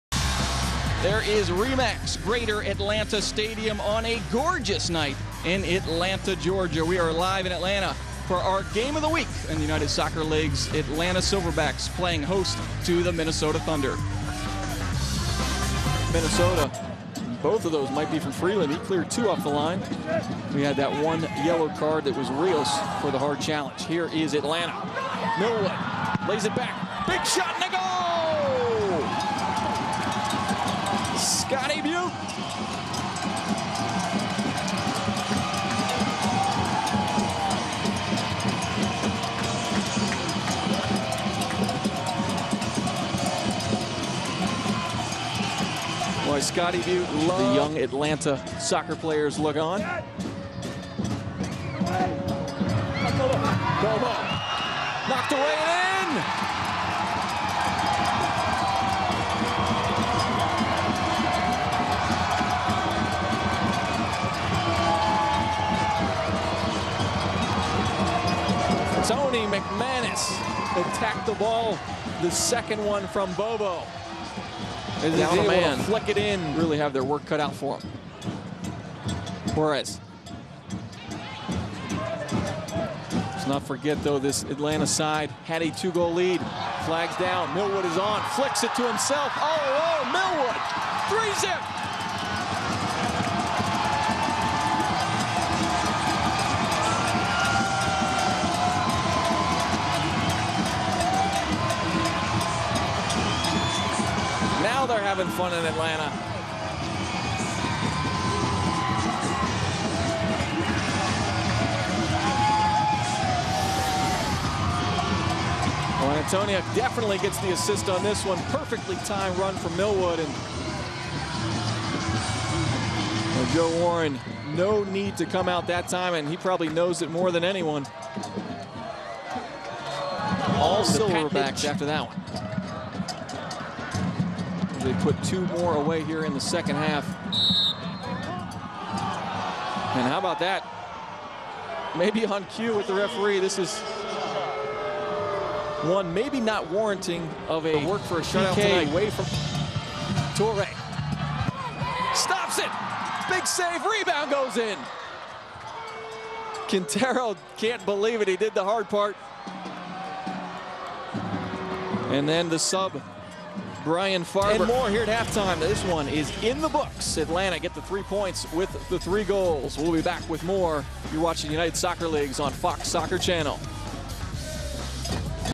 USL: Minnesota at Atlanta 4/27/2007
Highlights of the Fox Soccer Channel broadcast of a United Soccer Leagues First Division match between the Atlanta Silverbacks and Minnesota Thunder played on April 27, 2007 at RE/Max Greater Atlanta Stadium.